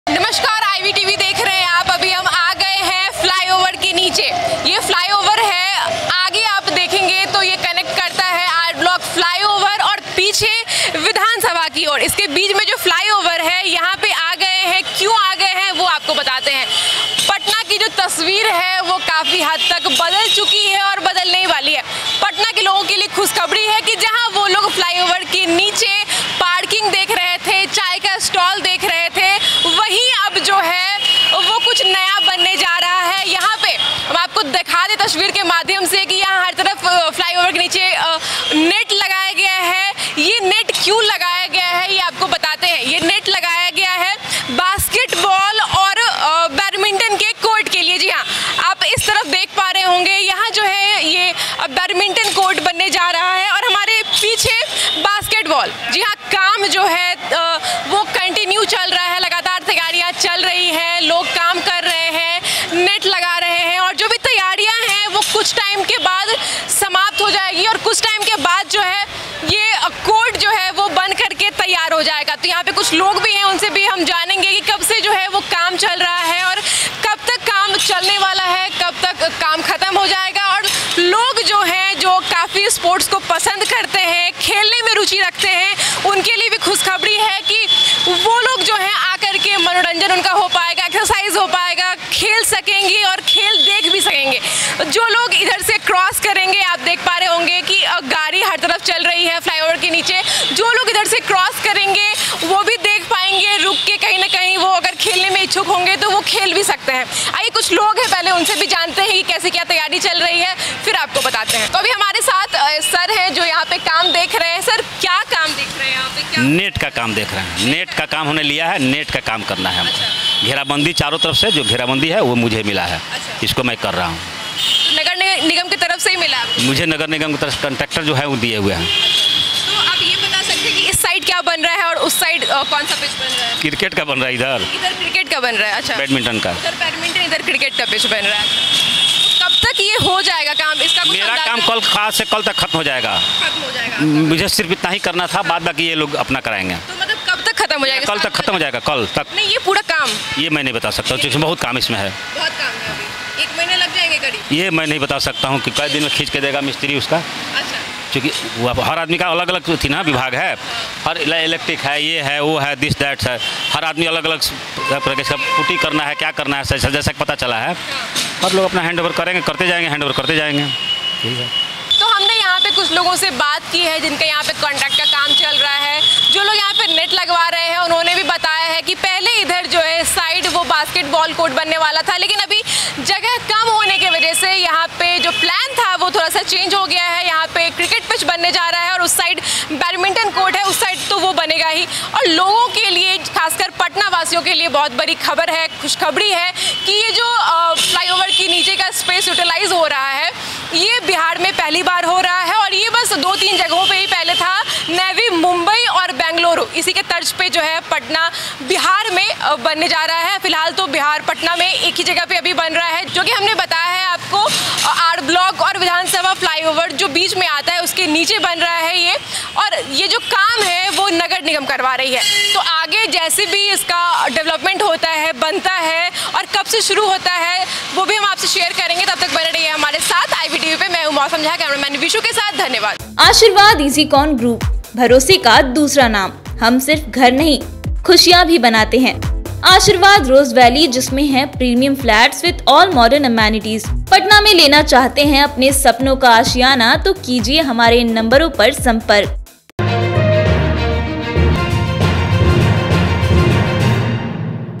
Mumbai की तरह बनेगा Patna ! Flyover के नीचे शुरू हो गया ये काम, देखिए Mausam Jha की ग्राउंड रिपोर्ट
मुंबई की तरह बनेगा पटना ! फ्लाईओवर के नीचे शुरू हो गया ये काम, देखिए मौसम झा की ग्राउंड रिपोर्ट..